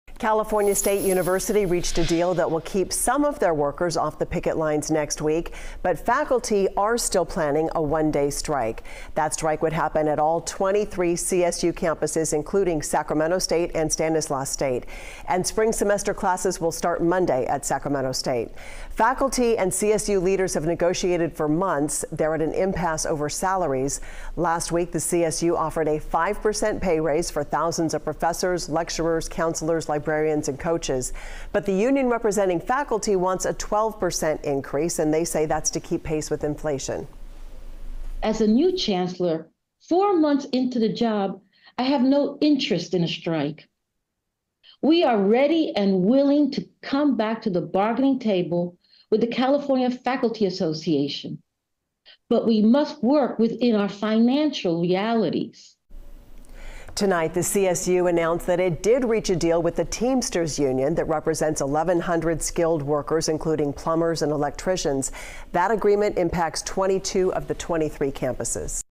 CSU averts strike after reaching deal with Teamsters Union
Skilled trade workers with the California State University have come to an agreement on wages, effectively calling off a planned strike, officials said Friday.  The CSU announced the tentative agreement with the Teamsters Local 2010 Union, according to a CSU spokesperson.